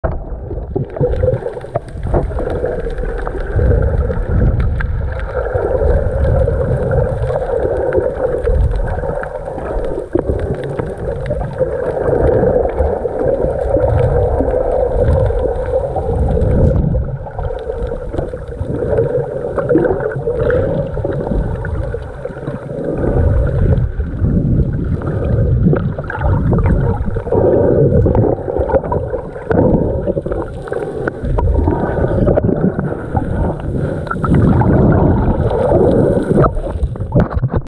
Snorkeling Ahihi-Kinau Natural Area Reserve
Awesome spot with tons of fish and coral, just no turtles